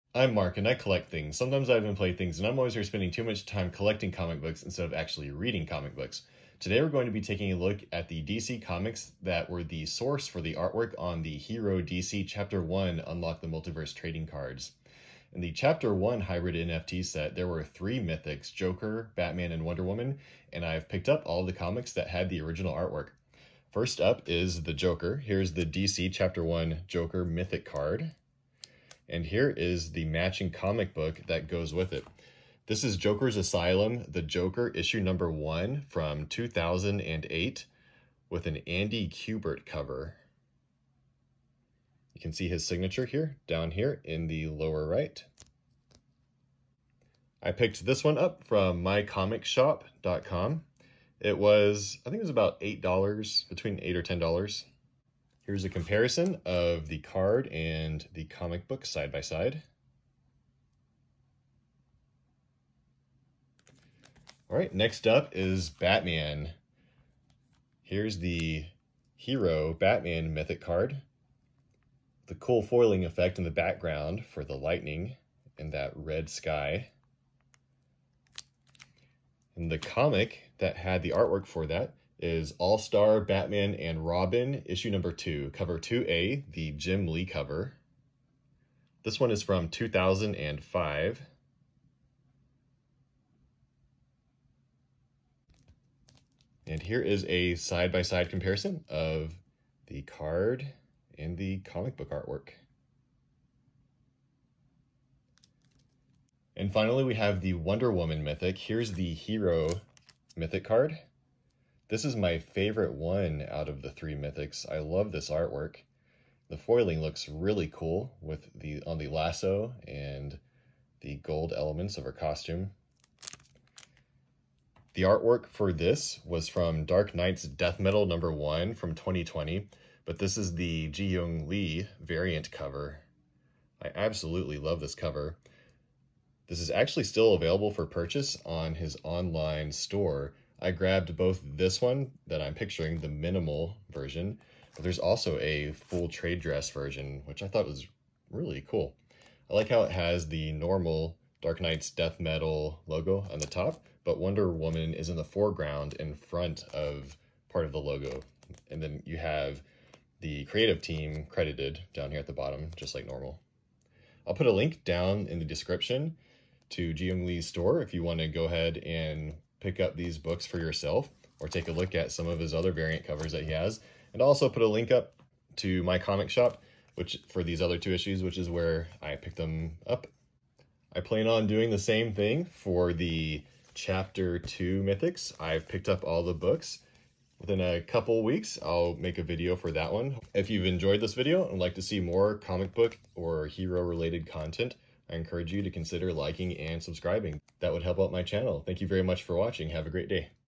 Showing Comic Covers for Mythic Hro DC Chapter 1 Hybrid NFT Trading Cards Unlock the Multiverse
In this video we show the comics that were the source art for the 3 mythic rarity cards in Hro DC Chapter 1.

0:00 Intro
0:24 Joker - Andy Kubert
1:05 Batman - Jim Lee
1:46 Wonder Woman - Jeehyung Lee
3:00 Outro